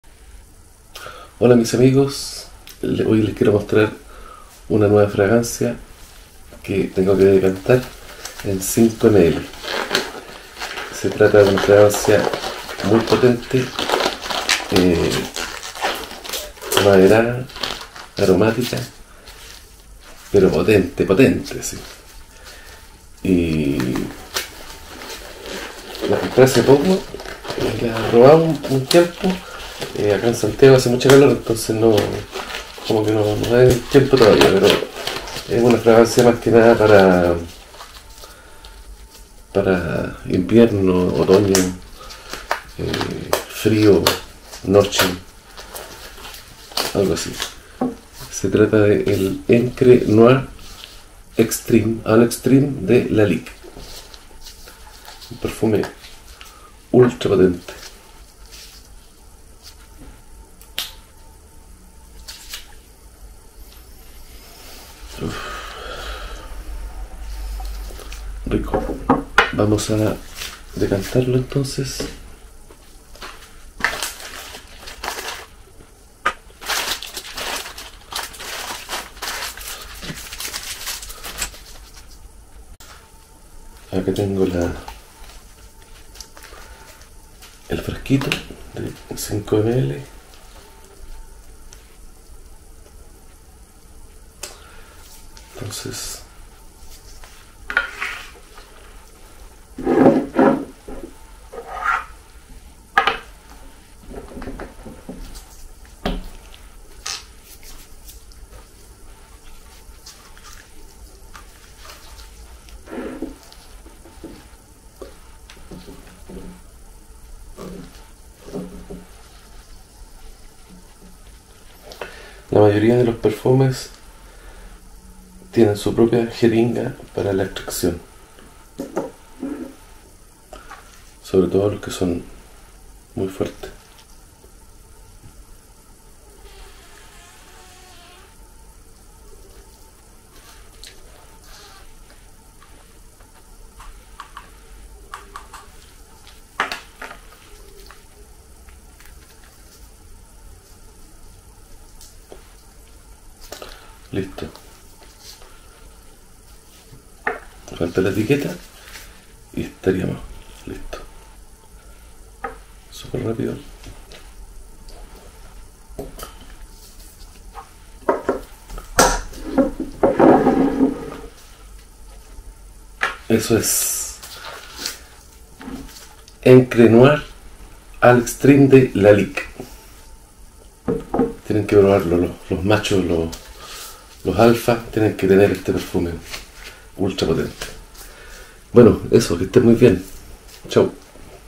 Entre Noir Al’Extreme de la casa Lalique llenado Decants 5ML
.Entre Noir Al’Extreme de la casa Lalique es una fragancia de la familia olfativa Amaderada Aromática para Hombres
Decants disponibles en 5ml y 10 ml.
Consulta directo, también disponibles en mercado libre.